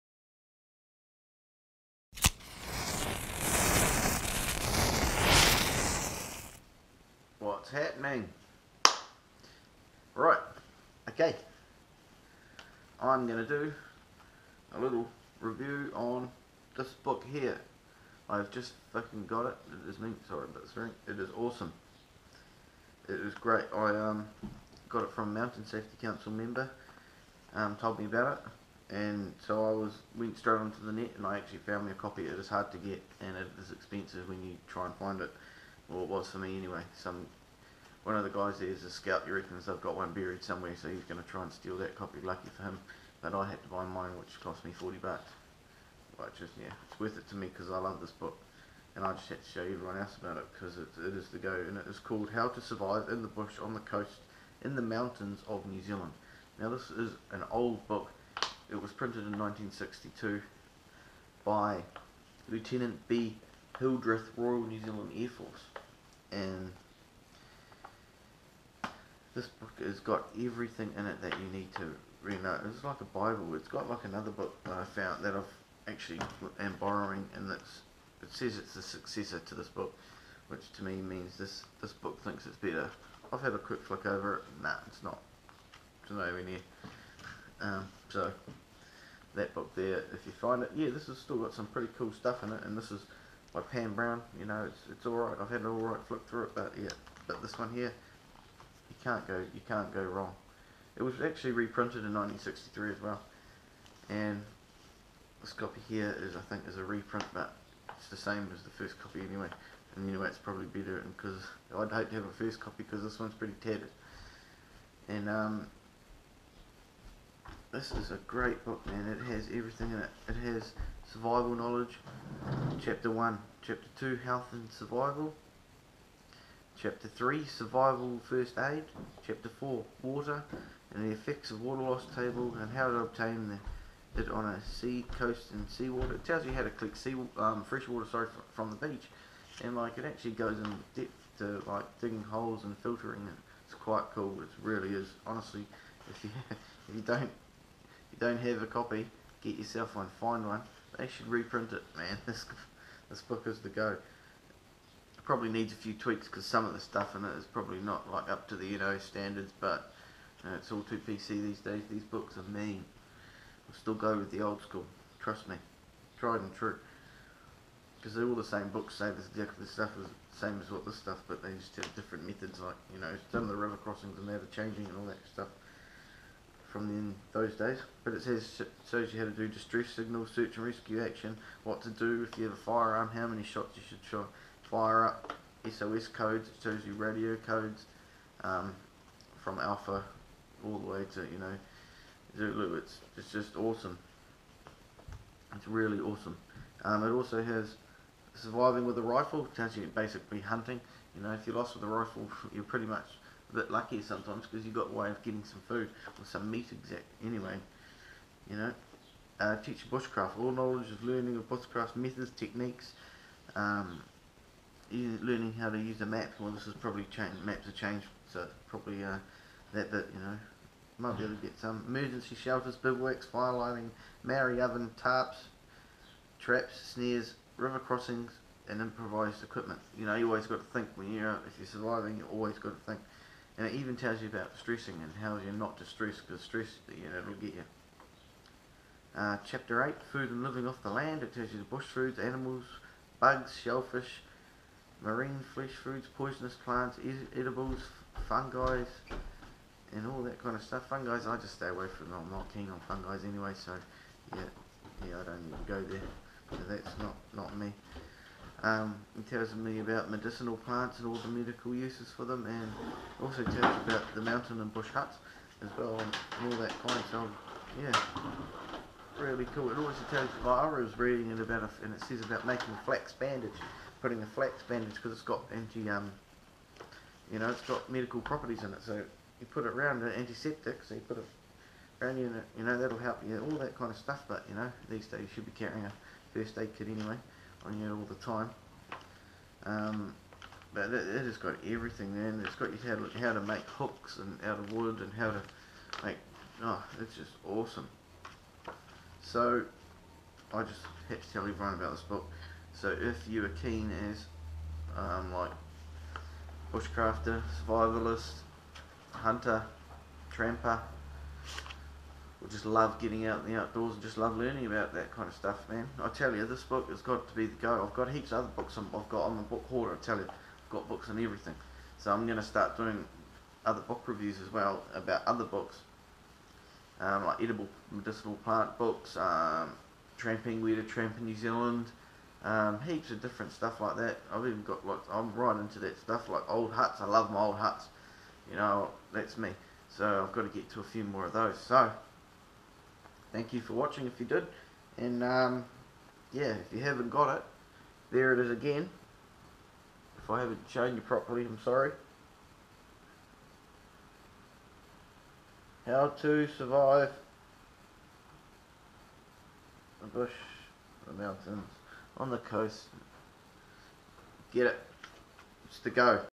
How to survive New Zealand
This book is a bible for the New Zealand bush man its old and hard to find but a very good read i know a guy that tramped the hole southern alps with this book by his side